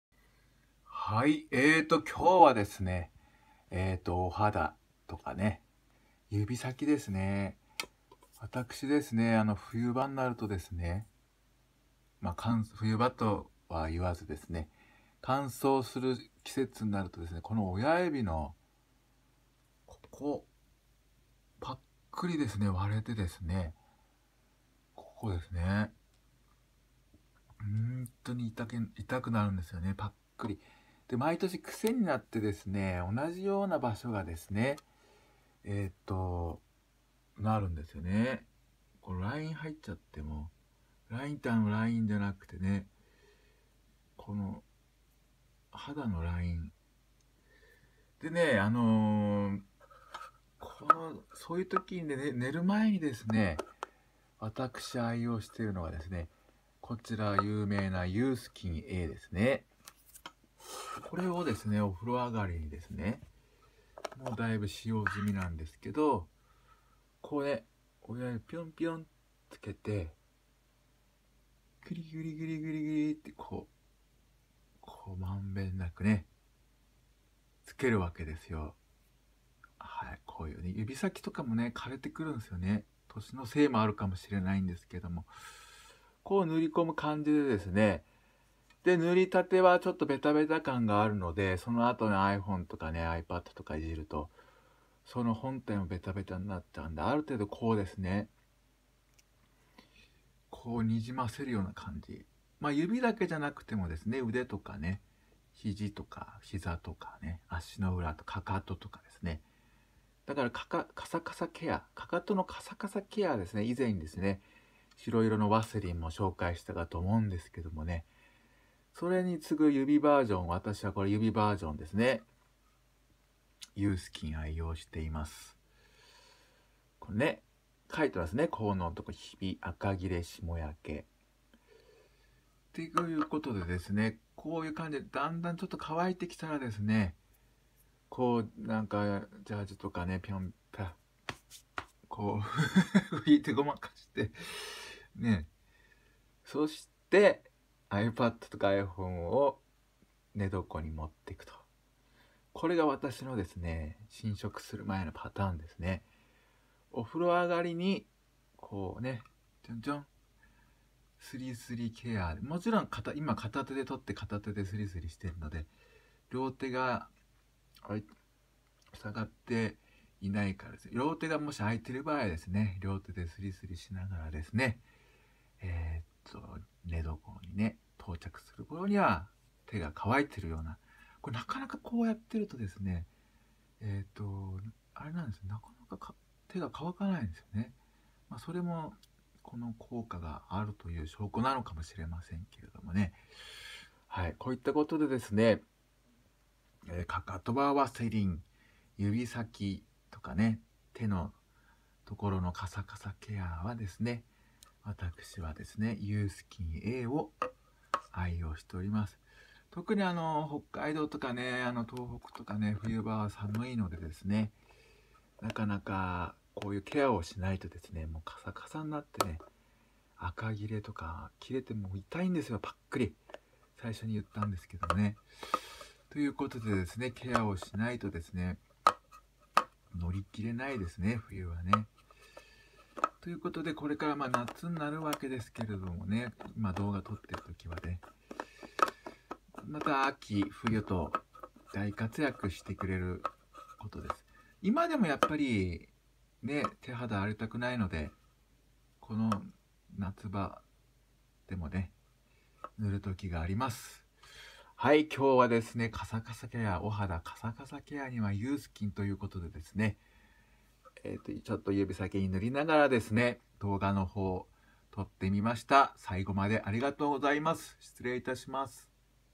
手指ひび割れクリームユースキンa口コミした
乾燥する季節に手肌保護クリームとして愛用のユースキンaの使用して非常に良かったです。なので口コミでも評価の良いこのクリームを紹介します。特に北海道の冬場は壮絶に指が荒れます。ひび割れもなります。わが家になくてはならないアイテムです。最強です。ありがたや。